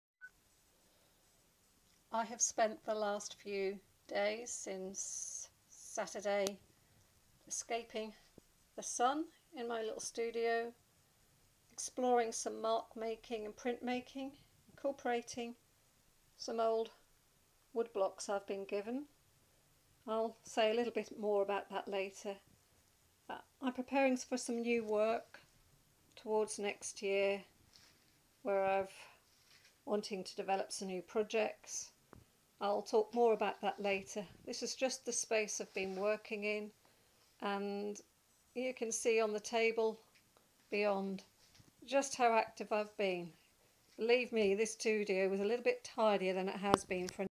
17 July 2018
Working in the studio to escape the heat. Exploring some ideas surrounding the landscape and marks in the landscape. Looking at some of my silkscreen prints and combining with some painted and printed cloth. I have been incorporating some old woodcuts many unfinished donated to me for use by the family of the maker (will reveal more later if these develop into something) Lot more work to do yet..need to go out and draw the landscape.